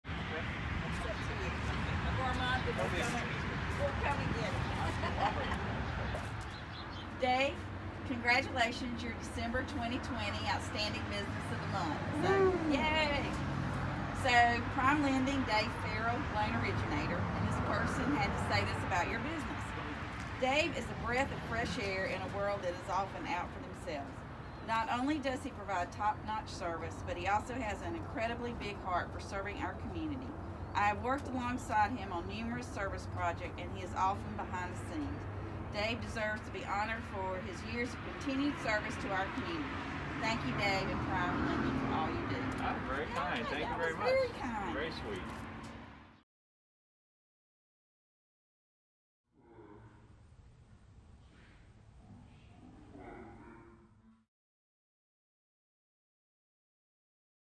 CCOC Outstanding Business of the Month: PrimeLending, Dave Ferrell - December 2020
Congratulations to the December 2020 Outstanding Business of the Month: PrimeLending - Clayton, Dave Ferrell. Dave and the team at PrimeLending were nominated for this award due to their devotion to our community and top-notch service.